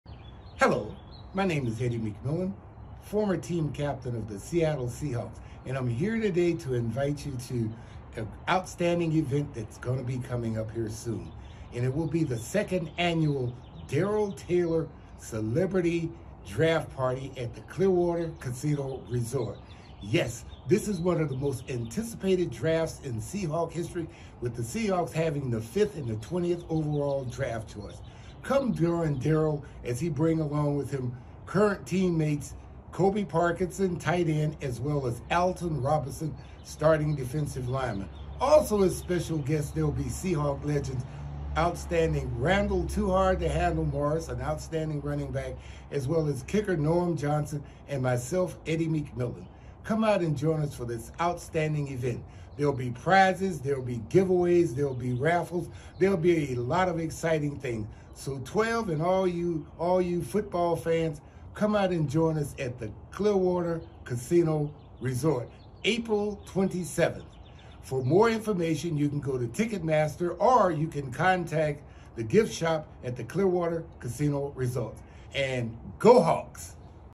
2nd Annual Football Draft Party - Hosted by Seattle player Darrell Taylor, DE
Join Darrell Taylor, DE and friends on Thursday, 4/27 and meet some current and former Seattle football players, get autographs and photos, and watch the 1st round of the Football Draft! Plus, enjoy fun raffles, Trivia with prizes, a silent auction and MORE!